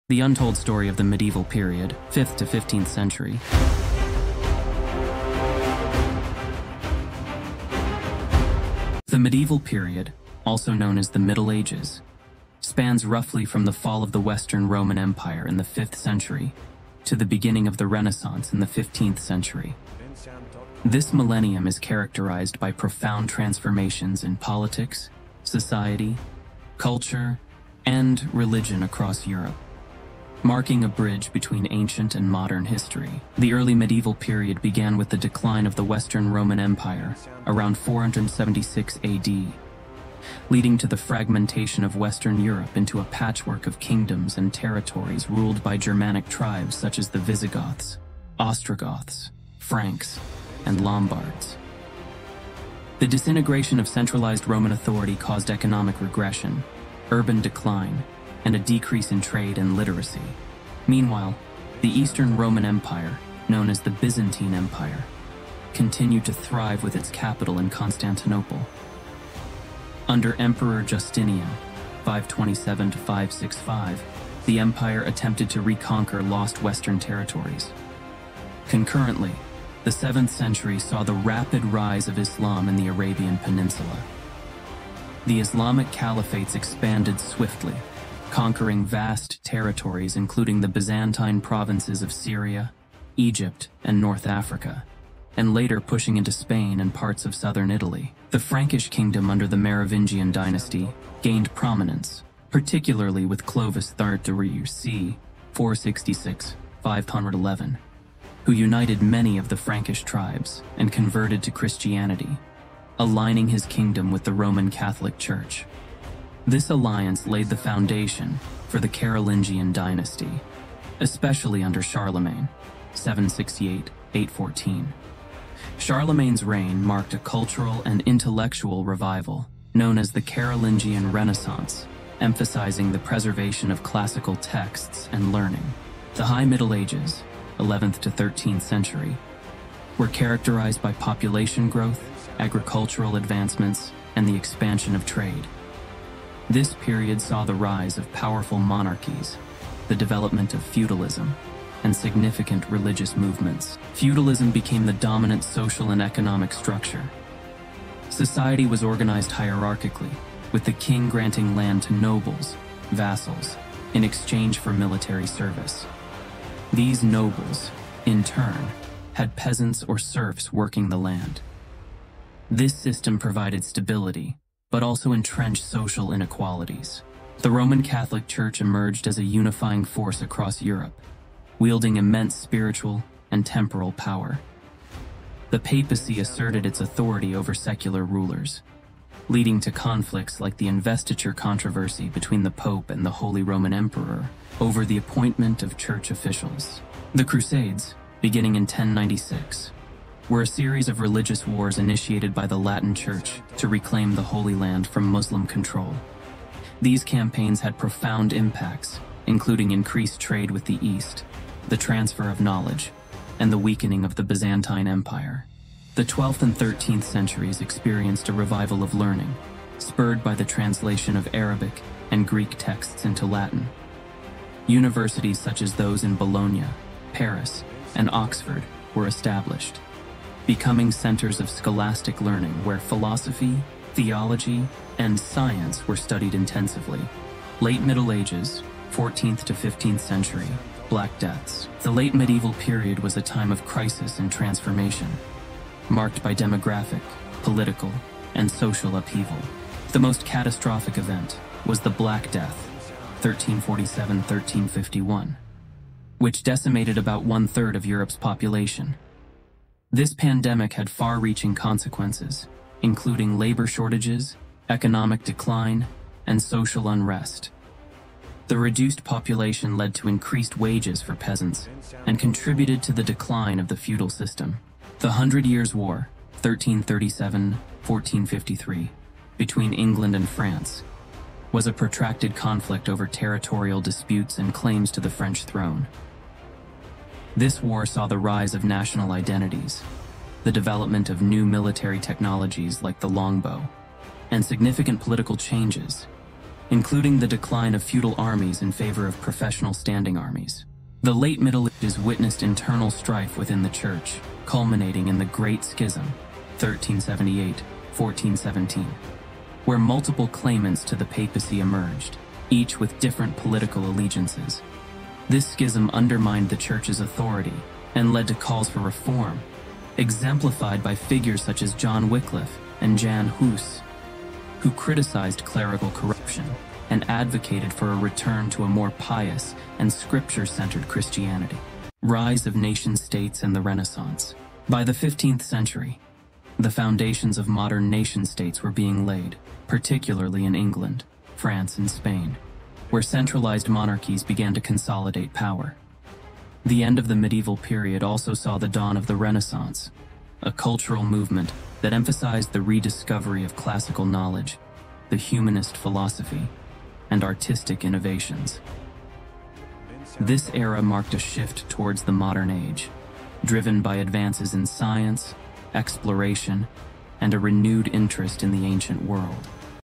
The Medieval Period (5th to 15th Century) | The Untold Story of Medieval Ages| With English Subtitle
Overview of the Medieval Period (5th to 15th Century)

The medieval period, or Middle Ages, spans from the 5th century fall of the Western Roman Empire to the 15th century Renaissance. This era saw significant changes in politics, society, culture, and religion in Europe, bridging ancient and modern history.

Early Middle Ages (5th to 10th Century)

After the Western Roman Empire fell in 476 AD, Europe fragmented into various kingdoms ruled by Germanic tribes. The Byzantine Empire thrived in the East, and the rise of Islam in the 7th century led to significant territorial expansions by the Islamic Caliphates. The Frankish Kingdom grew powerful under leaders like Clovis I and Charlemagne, the latter fostering a cultural revival known as the Carolingian Renaissance.

High Middle Ages (11th to 13th Century)

This period featured population growth, agricultural advancements, and expanded trade. Feudalism became the dominant social system, with the Roman Catholic Church wielding significant influence. The Crusades began in 1096, aiming to reclaim the Holy Land, and universities emerged as centers of learning, promoting philosophy, theology, and science.

Late Middle Ages (14th to 15th Century)

Marked by crisis and transformation, the late medieval period saw events like the Black Death, which killed about one-third of Europe's population, leading to economic and social changes. The Hundred Years' War between England and France fostered national identities and new military technologies. The Church faced internal strife during the Great Schism, prompting calls for reform from figures like John Wycliffe and Jan Hus. By the 15th century, centralized monarchies began to form in countries like England, France, and Spain, setting the stage for the Renaissance, which emphasized classical knowledge, humanist philosophy, and artistic innovation, marking the transition to the modern age.